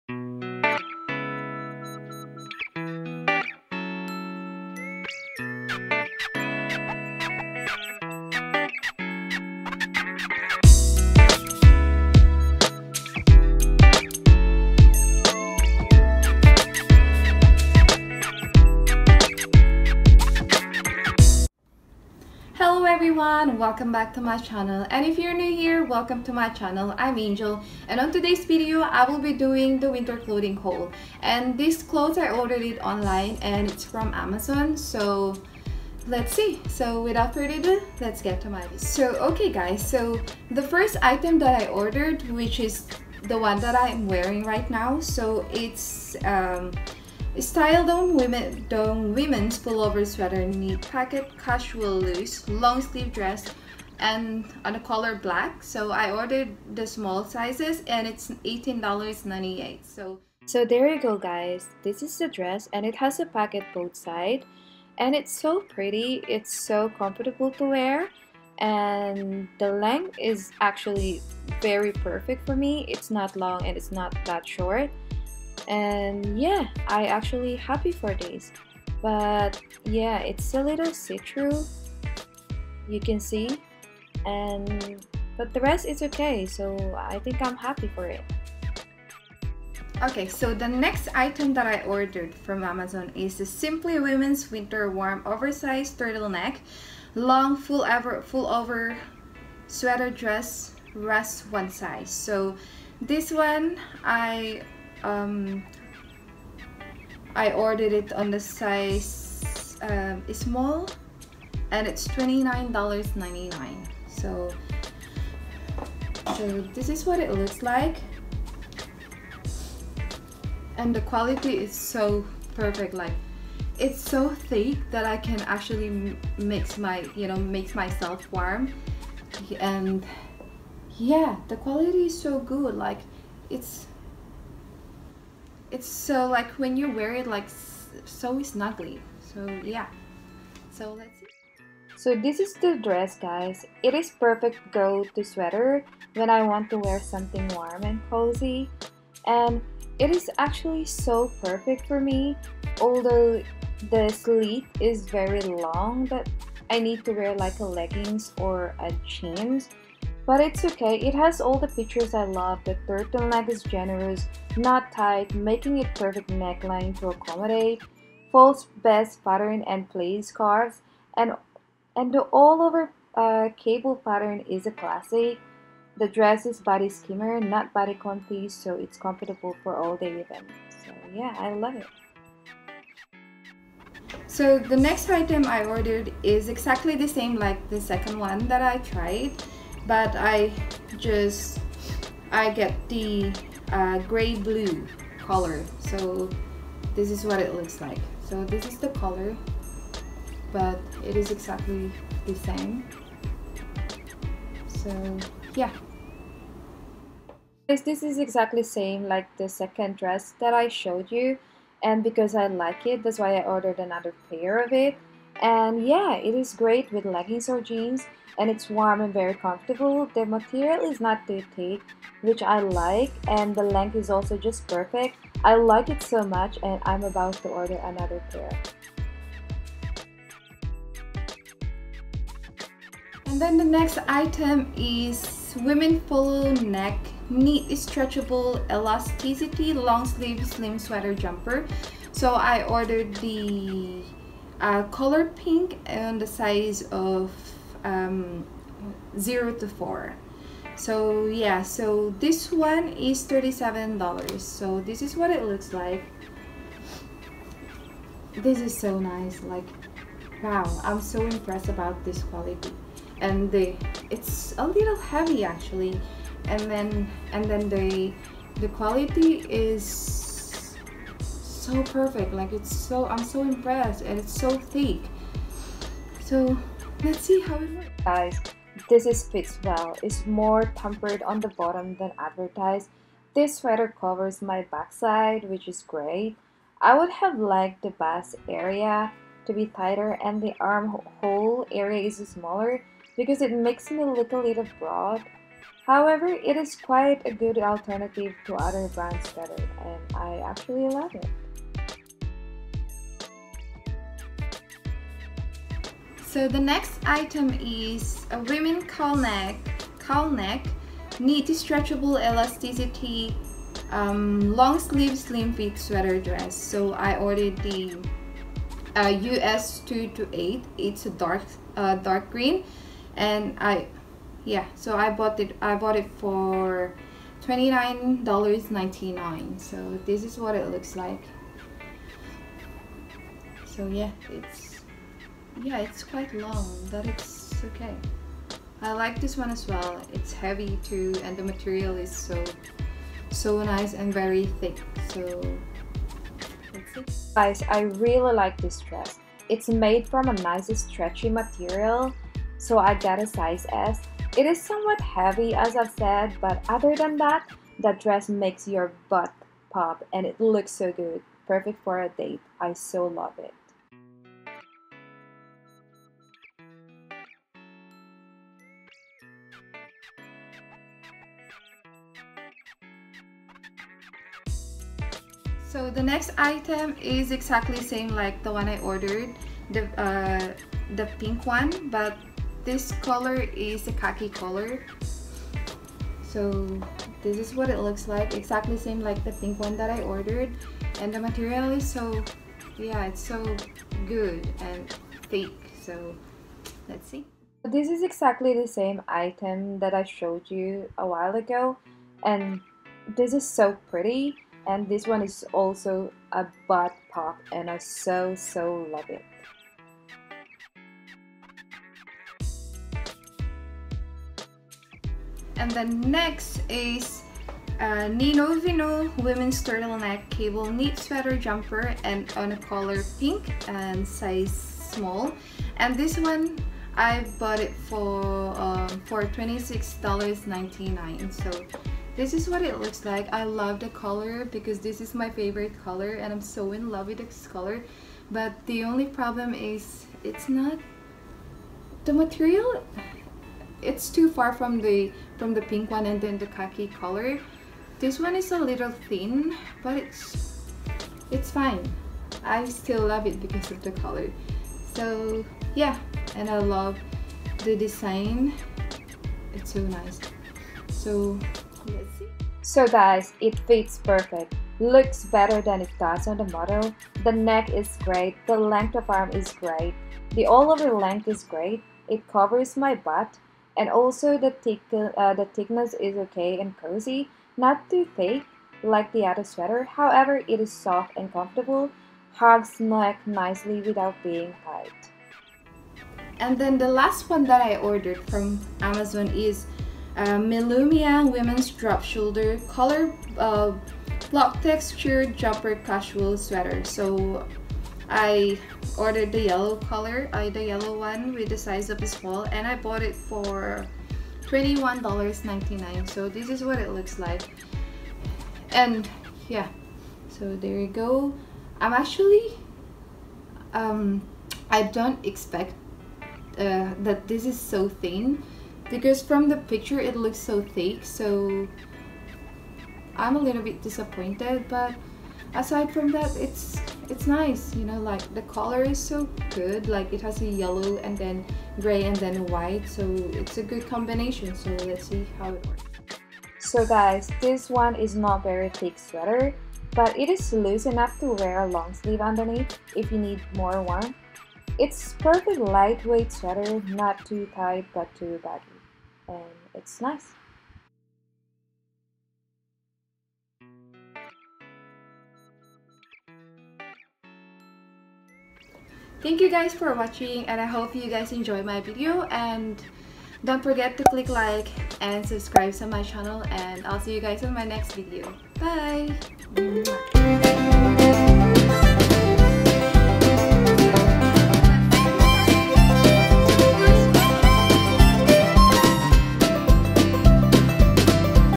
WINTER CLOTHING HAUL FROM AMAZON |angelvlogs
Its been a hot minute since my last haul, but now I'm finally ready to share new pieces that i added into my wardrobe!